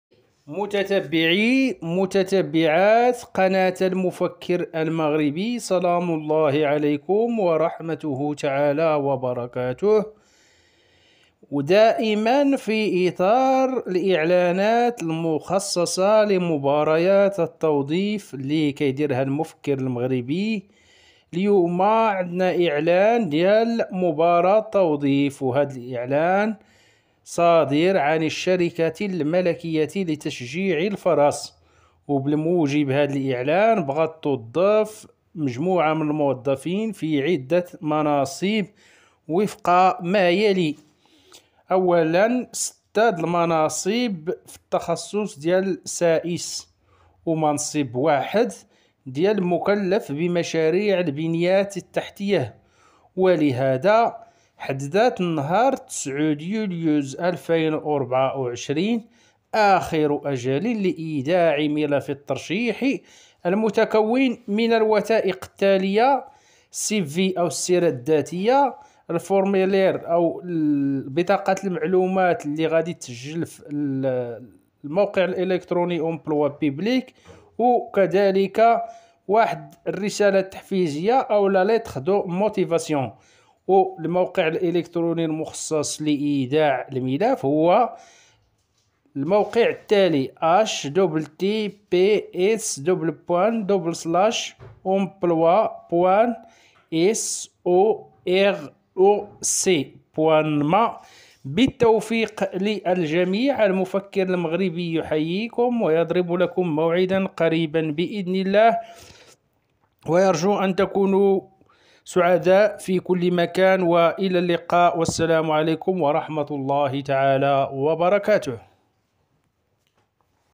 كونكور_جديد:التوظيف في المؤسسات:الشركة الملكية لتشجيع الفرس تعلن مباراة توظيف#الشروط#المسطرة#الملف
كونكور_جديد:التوظيف في المؤسسات: الشركة الملكية لتشجيع الفرس تعلن مباراة توظيف#الشروط#المسطرة#الملف#الاجل#الوثائق#الدبلوم#الشواهد#العمل#الخدمة#بالتوفيق_للجميع